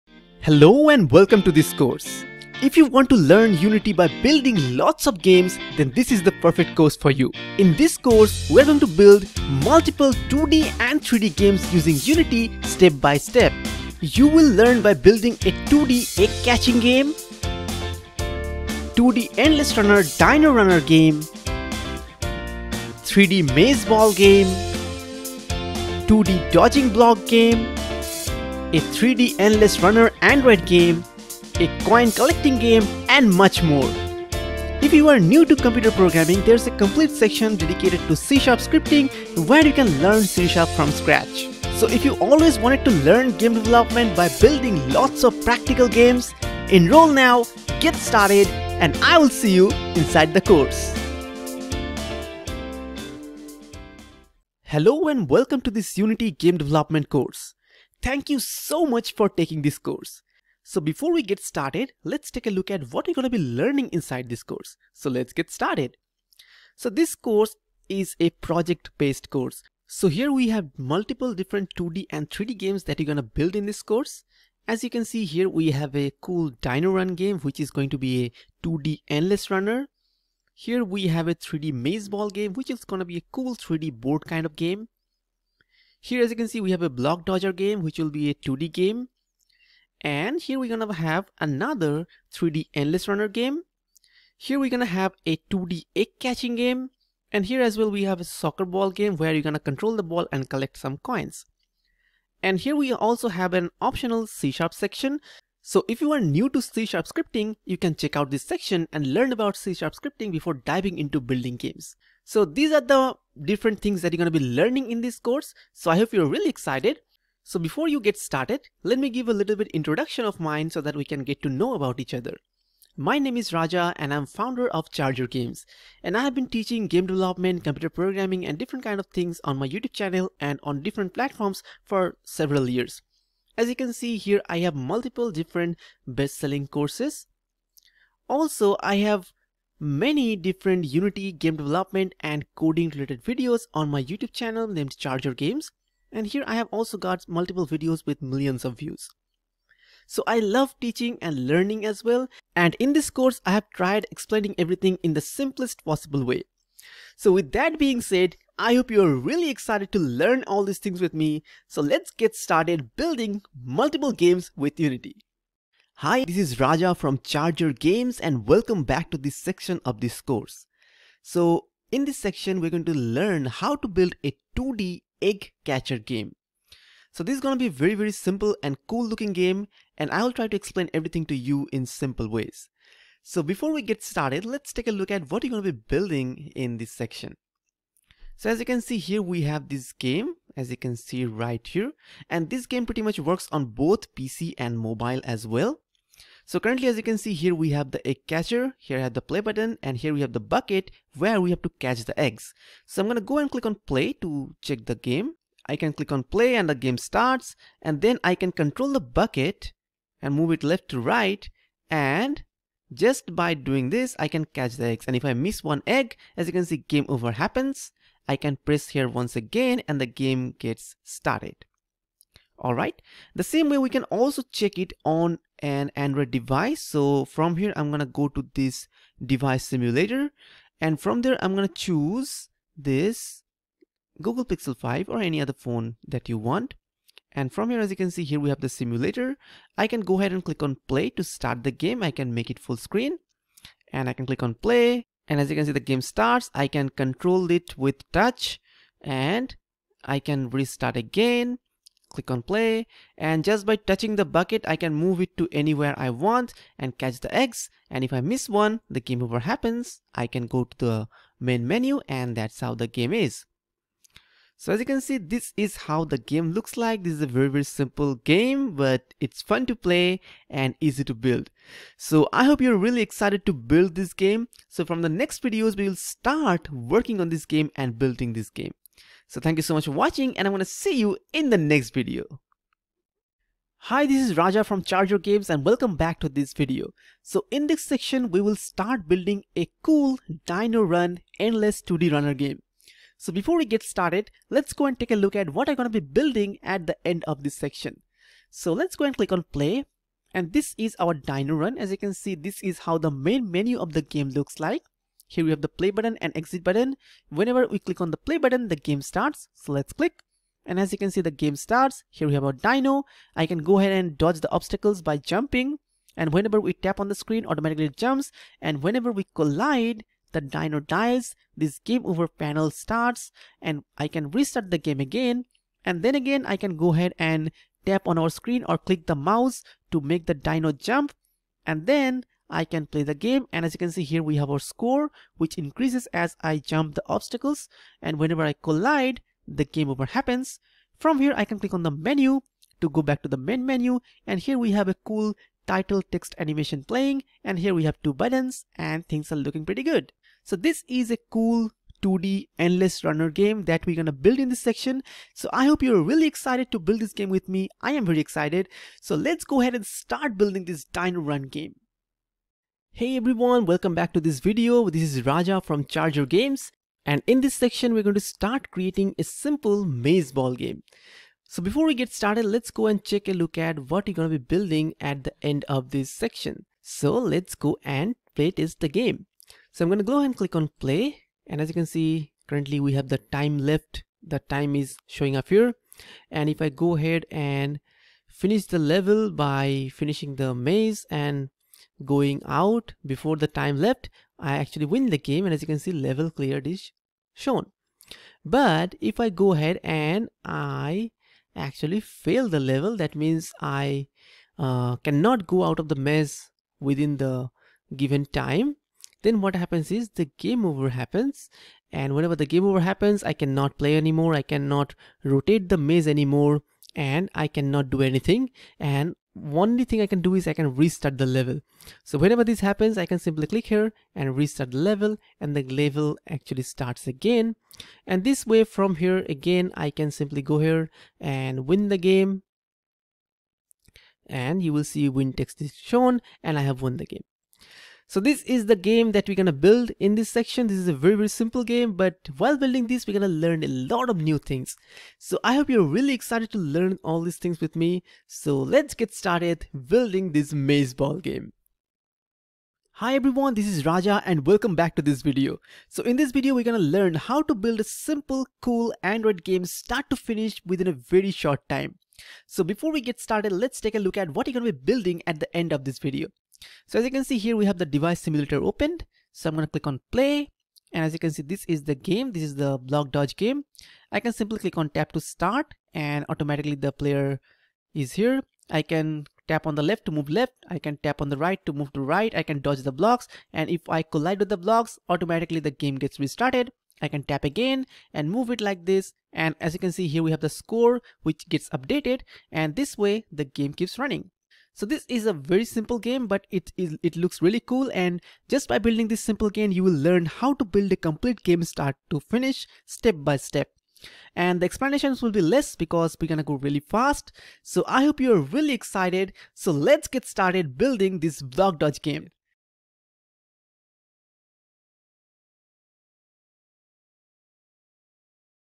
Unity 2023 Game Development : Build 6 Games with Unity 2023 [ New Course ]
(12+ Hours of Video Content )

Learn Unity Game Development : Build 6 Games with Unity 2023

List Of Games You Will Build in this Course:

1. 2D Egg Catching Game

2. Block Dodging Game

3. Dino Run Game

4. 3D Maze Ball Game

5. 3D Endless Runner Android Game

6. 3D Ball Coin Collector

In this course You will learn Unity Game Development and C# Scripting by building Multiple different 2D & 3D Games with Unity & C#. You will also learn to build and export your Games to Android Mobile Platform. This course is great for developers who wish to learn Unity Game development and C# Scripting by building real world example projects.

You will start by installing and Setting up Unity . Then you will start building different 2D & 3D Game Projects with Unity & C#.

There's a C# Scripting section for beginners as well. If you're new to C#, you can watch these videos before diving into Game Development with Unity.

Here's the list of things Your will Learn :

Build 6 Different Games with Unity

Learn C# by Building Real Games

Create Multiple 2D & 3D Games with Unity

Design Beautiful Animated Main Menu

Build A Portfolio of Different Game Projects

Learn C# Scripting from Scratch

Take this course and start building your own Games Today .

Arts from Gameart2d